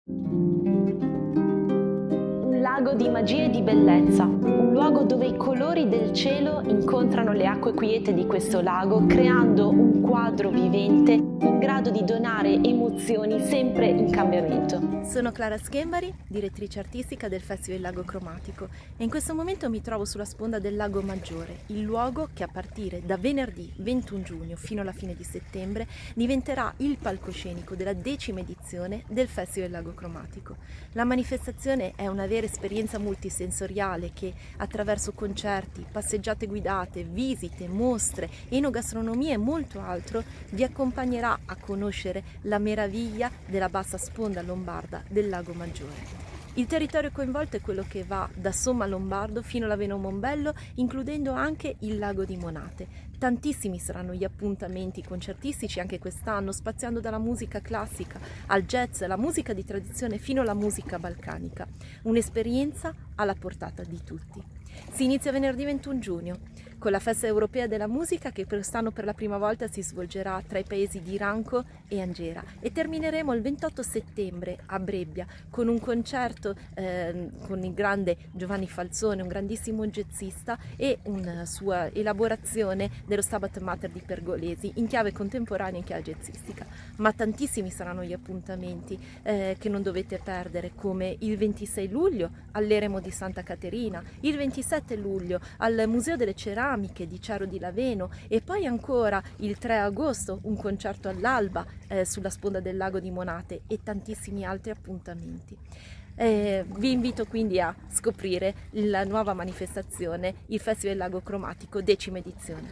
Varese - Festival Lago Cromatico, emozioni dal Maggiore
È giunta la decima edizione del Festival Il Lago Cromatico. Una rassegna che dal 21 giungo fino alla fine di settembre percorrerà tutta l'estate e soprattutto toccherà le più belle località del Lago Maggiore sulla sponda varesina. Un cammino di suoni, colori e sapori come solo questo specchio d'acqua sa regalare. Curato dalla direzione artisticha di Clara Schembari, il calendario è consultabile sul sito dell'evento.